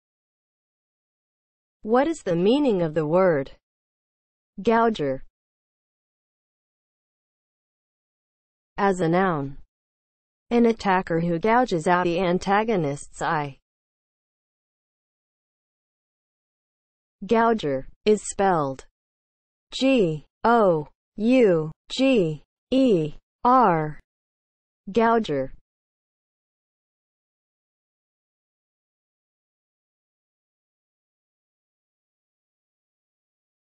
What is the meaning of the word GOUGER?
Definition and spelling of the word GOUGER.

(00:00) Intro
(00:07) As a noun
(00:15) Spelling

As a noun (00:07):
• an attacker who gouges out the antagonist's eye